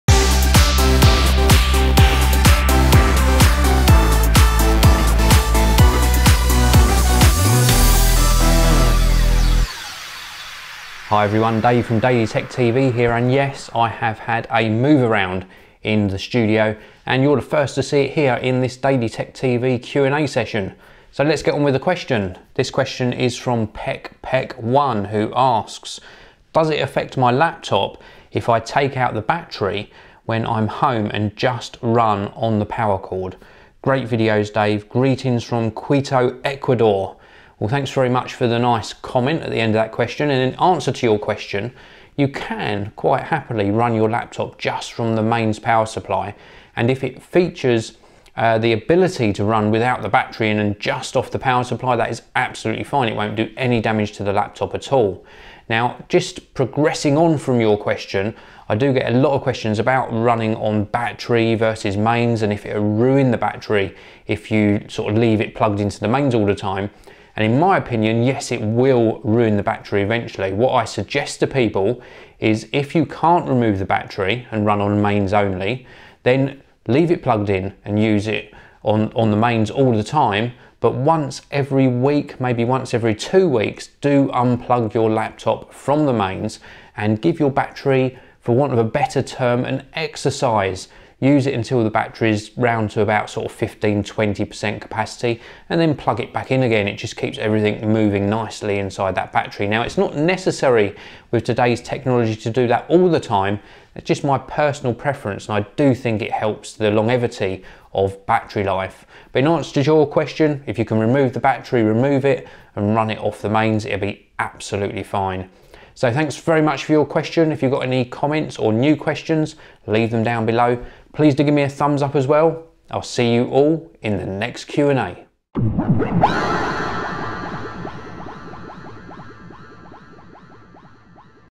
Batteryless Laptop
Batteryless Laptop ... is it safe to remove your battery & run of the mains power supply? Check it out in this video.